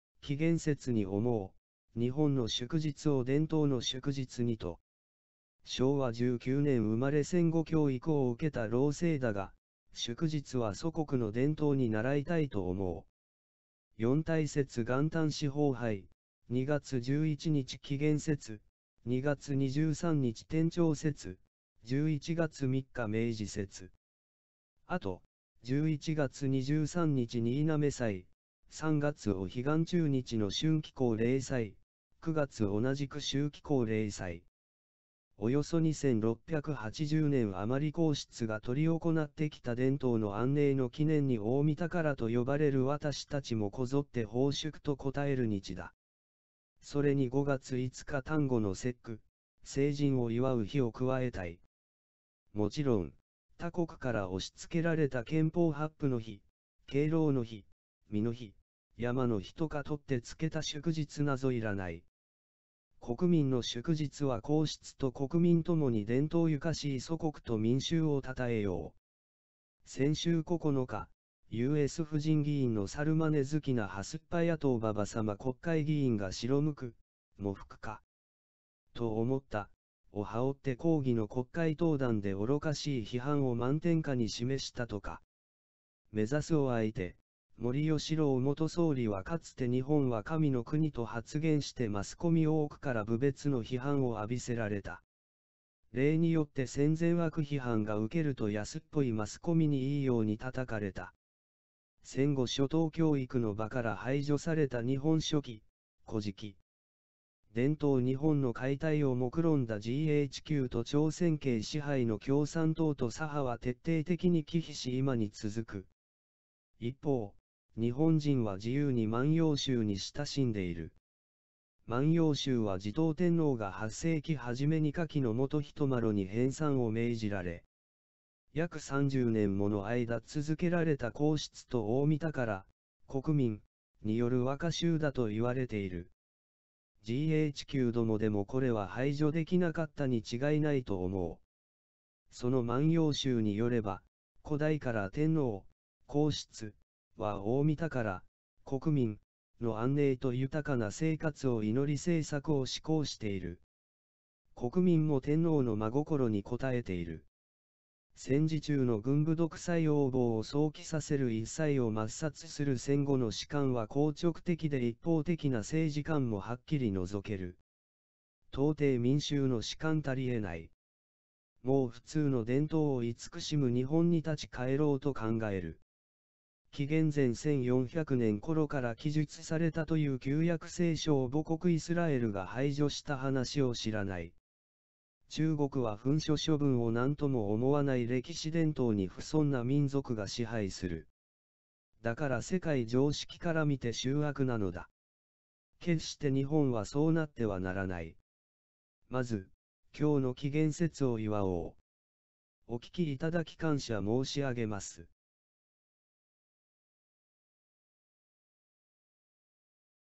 紀元節に思う、復古意識ではない伝統の祝日にと…
昭和19年生まれ戦後教育を受けた老生ですが、祝日は祖国の伝統に倣いたいと思う。
「四大節」元旦＝四方拝、2月11日＝紀元節、2月23日＝天長節、11月3日＝明治節。後、11月23日＝新嘗祭、3月お彼岸中日の春季皇霊祭、9月同じく秋季皇霊祭…